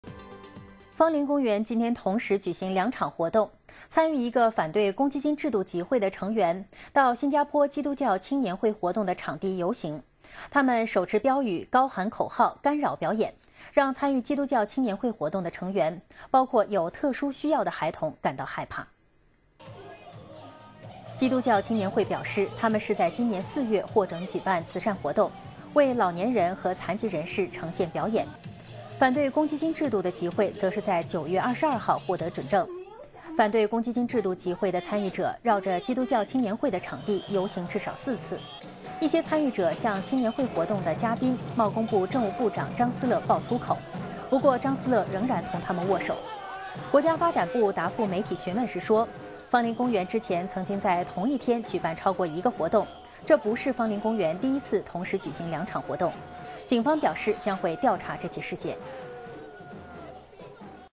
芳林公园骚动 部长张思乐遭抗议者爆粗口
芳林公园今天有两场活动同时举行，结果酿成一场骚动。一组抗议公积金活动的参与者到基督教青年会&nbsp;(YMCA)&nbsp;嘉年华的场地游行。